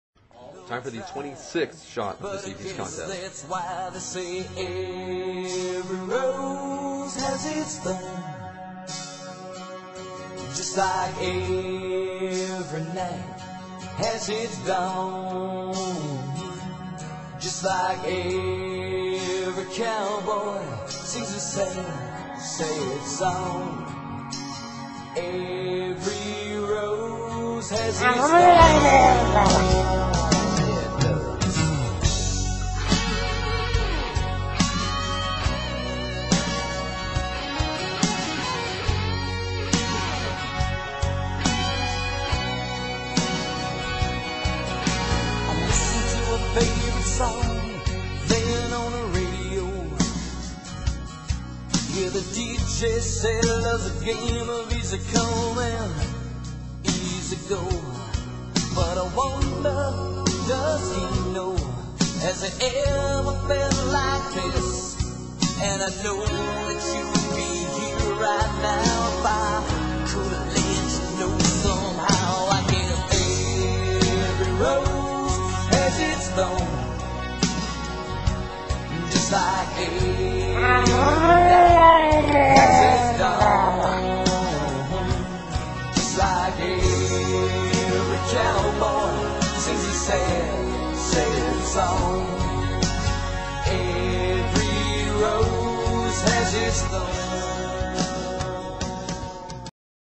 Jesse Pukes
A power hour drinking contest.  One contestant down.